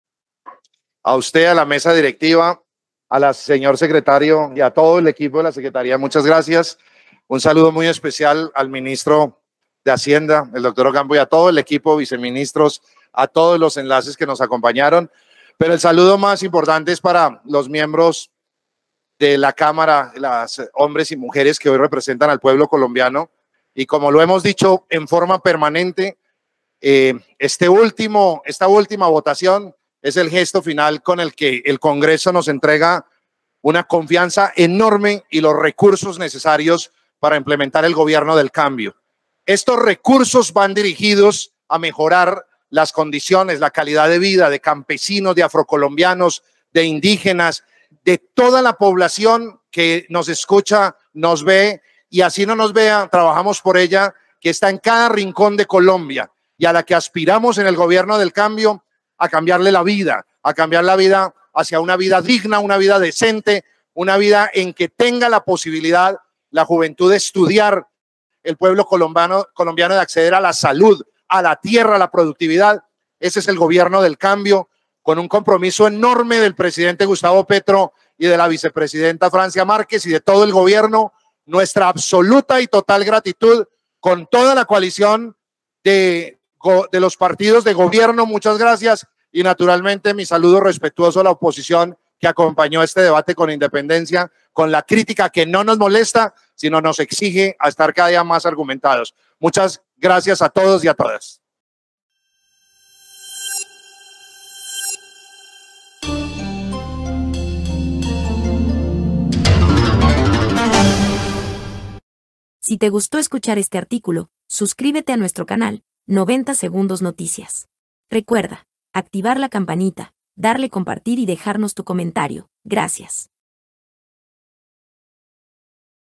Ministro Alfonso Prada agradece el “gesto final “al aprobar la conciliación de la Reforma Tributaria
90 Segundos Noticias, Somos independientes, con neutralidad e imparciales, objetivos y honestos en su concepto estricto. Sin prejuicios, justos y equilibrados. ...